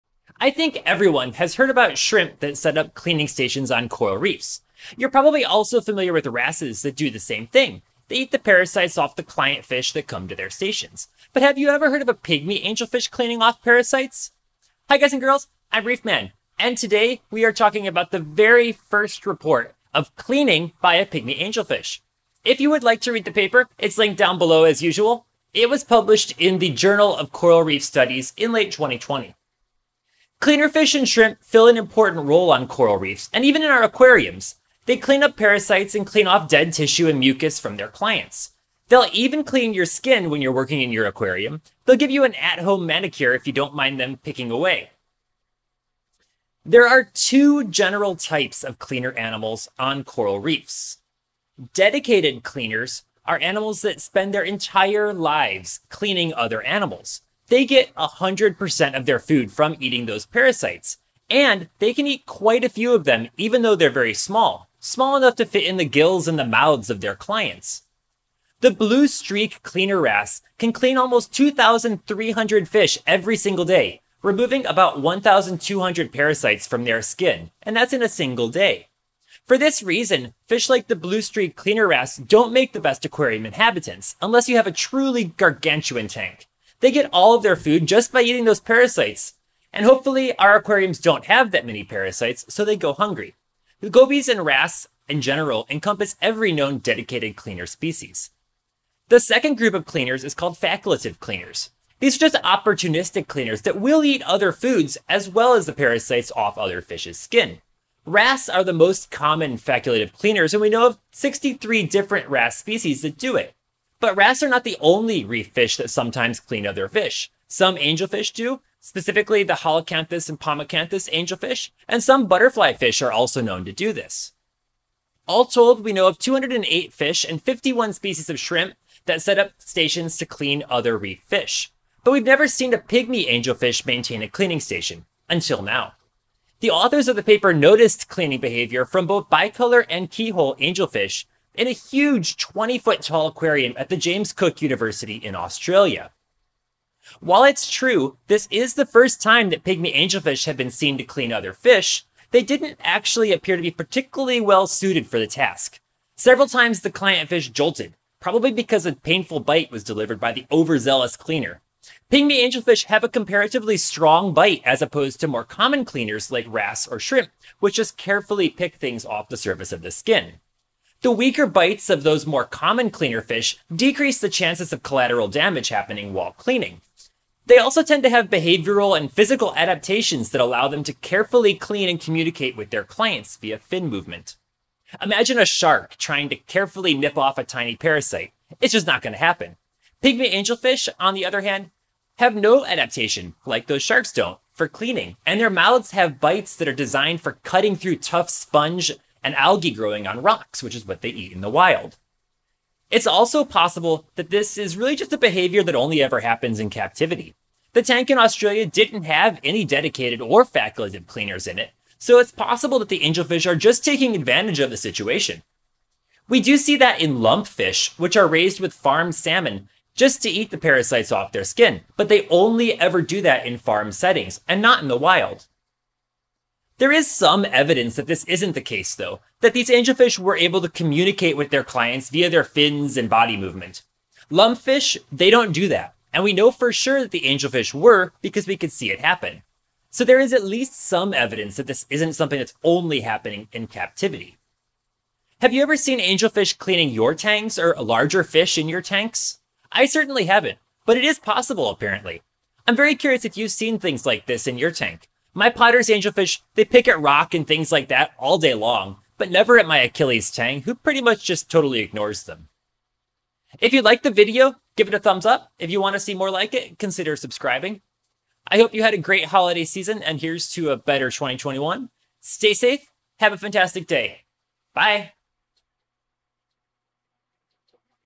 Pygmy Angelfish as Reef Cleaner Fish
Did you know Pygmy Angelfish liked the Bicolor Angelfish and Keyhole Angelfish could fill the role as a cleaner fish, removing parasites from other coral reef fish? Neither did anyone till just recently, when these fish were noticed cleaning Blue Tangs at the James Cook University! My Potter's Angelfish certainly show no interest in my tangs in my reef tank!

Cleaner shrimp make interesting inhabitants of reef aquariums, and generally won't pick too much on coral. They'll happily steal food from LPS though, so be careful if you have lots of mysis shrimp loving LPS in your reef tank. They'll also happily clean your hands while you're working in your tank, which feels a bit weird and probably slightly raises risk of infection - there's a lot of bacteria in your saltwater - just like in the ocean itself!

Abstract
Cleaner fishes remove ectoparasites, mucus and dead tissues from other ‘client’ organisms. These mutualistic interactions provide benefits for the ‘clients’ and, on a larger scale, maintain healthy reef ecosystems. Here, we report two species of angelfishes, Centropyge bicolor and C. tibicen, acting as cleaners of the blue tang Paracanthurus hepatus in an aquarium. This observation is the first time that pygmy angelfishes are recorded cleaning in any environment. This novel cleaning observation raises questions on the ecosystem role of cleaner fishes and which biological traits facilitate cleaning.

Filling an empty role: first report of cleaning by pygmy angelfishes (Centropyge, Pomacanthidae)
Pauline Narvaez, Renato A. Morais

Whale sharks Rhincodon typus get cleaned by the blue‐streak cleaner wrasse Labroides dimidiatus and the moon wrasse Thalassoma lunare in the Philippines